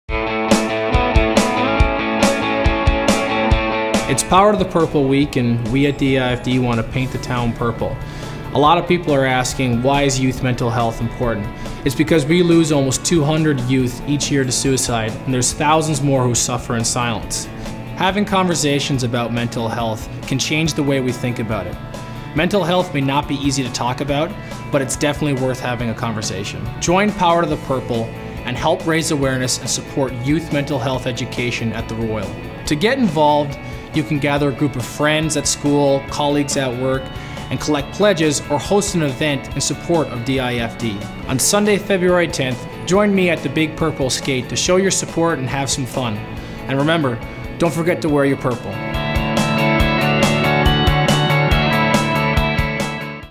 Colin Greening from the Ottawa Senators talks DIFD + Power to the Purple!
Take the 2013 Power to the Purple Challenge — an opportunity for people to show their support for youth mental health and break down the silence that is so often a barrier to getting help.

This year we are inviting schools, corporations and individuals in the community to raise awareness and funds in support of D.I.F.D. (Do It for Daron) and youth mental health education at The Royal.

This year Power to the Purple will run from February 1st to February 10th. We are inviting schools, corporations and individuals in the community to raise awareness and funds in support of D.I.F.D. and youth mental health education at The Royal.